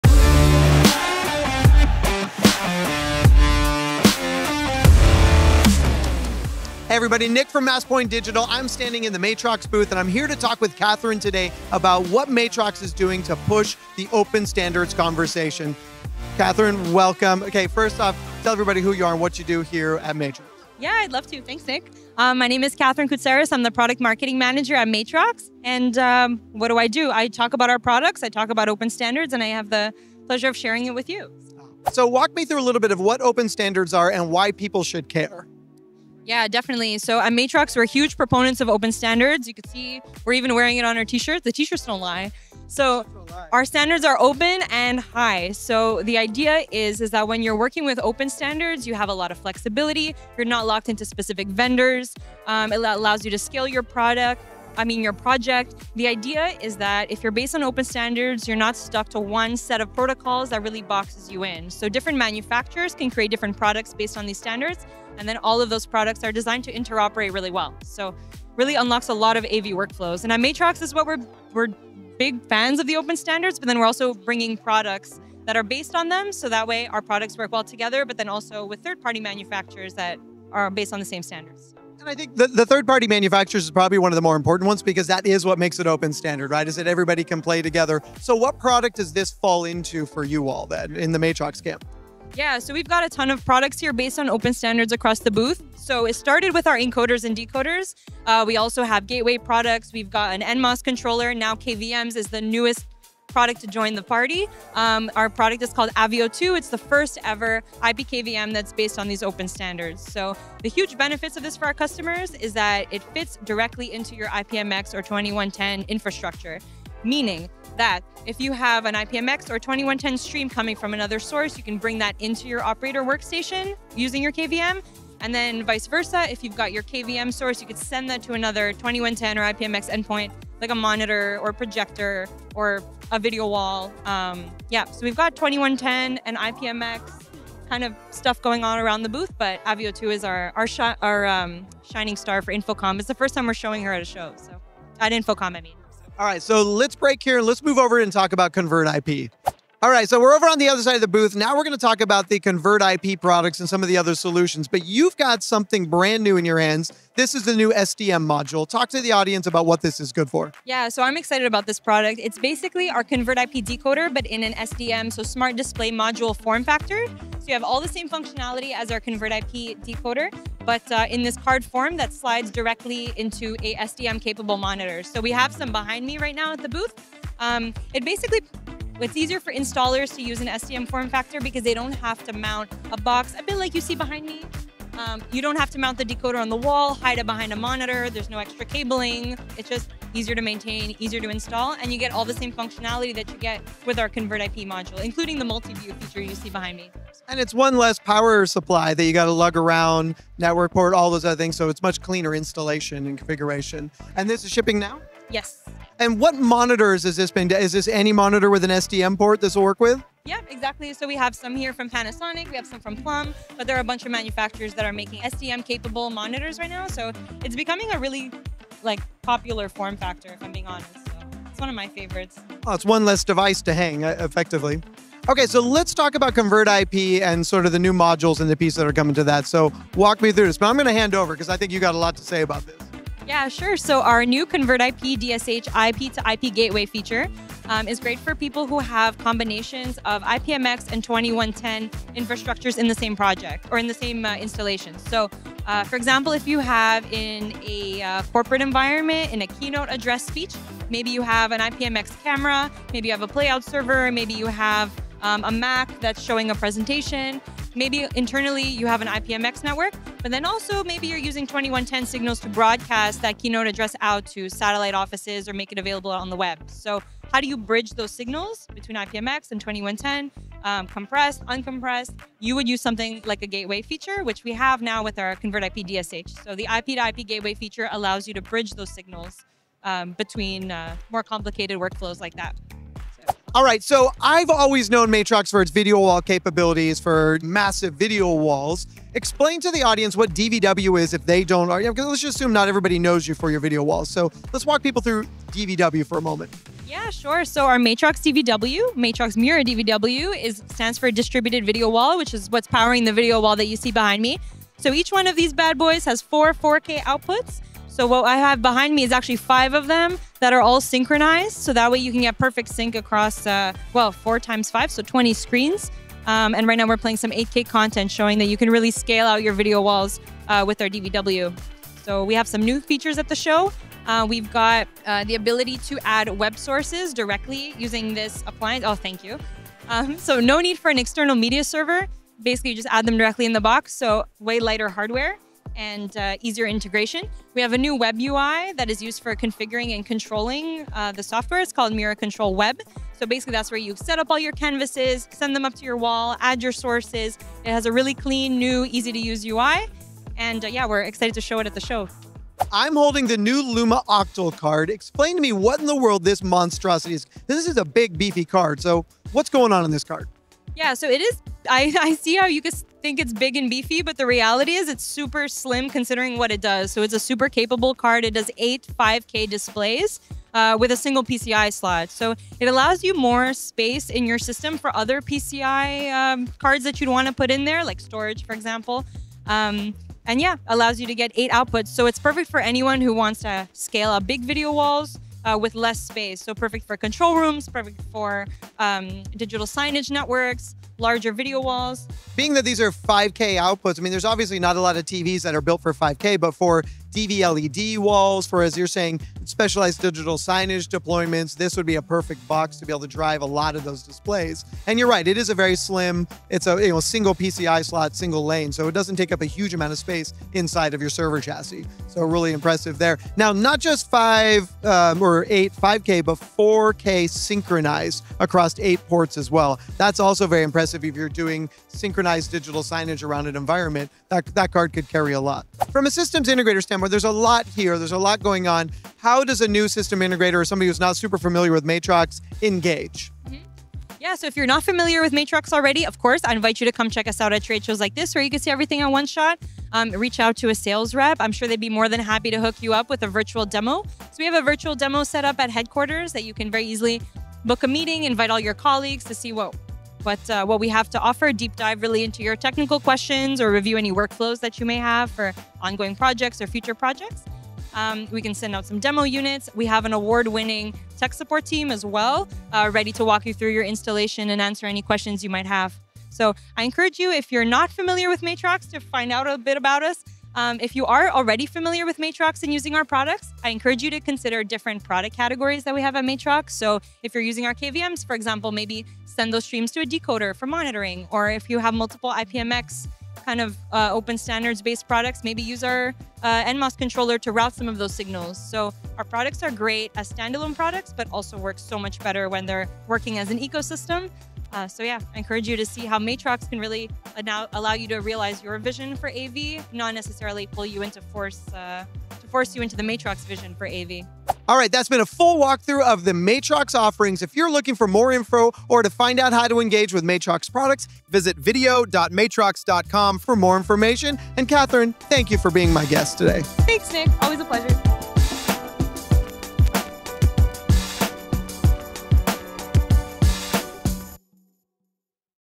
Matrox DEEP DIVE: Open Standards, Video Gateway Products, AND Video Walls! | Infocomm 2025
Matrox is making big moves in open standards and AV interoperability. In this in-depth booth tour from InfoComm 2025, we meet with Catherine Koutsaris to break down how Matrox is building an open, flexible ecosystem that spans IPMX, SMPTE ST 2110, and more.

You’ll see:

AVoIP with Open Standards: Learn how Matrox’s approach to IPMX and 2110 empowers integrators to avoid vendor lock-in while enabling truly interoperable AV workflows.

ConvertIP Platform: Explore the new SDM-form-factor ConvertIP decoder, perfect for clean installs into SDM-capable monitors, plus their brand-new IP-to-IP gateway feature for bridging 2110 and IPMX streams without extra boxes.

AVIO2 KVM: The first-ever IP-based KVM built on open standards, designed to plug directly into broadcast or enterprise networks using native IPMX or 2110.

Distributed Video Wall (DVW): A modular video wall system offering synced playback across 20 screens or more, with 4K outputs, onboard web sources, and a clean new web-based UI.

Luma Octal Graphics Card: Ultra-slim, high-density video output via a single PCIe slot, capable of driving eight 5K displays or synchronized 4K environments, ideal for LED control rooms and signage networks.

Whether you're designing scalable video walls, deploying interoperable AV networks, or just trying to clean up your install footprint, this deep dive into Matrox technology has the tools and thinking you need.

for more details.